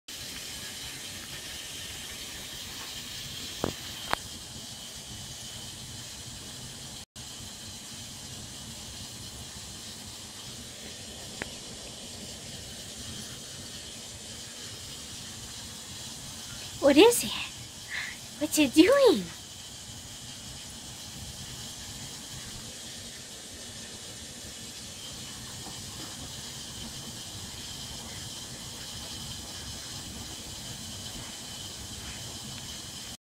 Standard Poodle Watches Washing Machine
Brizz likes to watch the clothes spin in the washing machine.

Do any of your pets like to watch any appliances or electronic devices?
(Brizz also likes watching the tv. I'll have to film that one too sometime)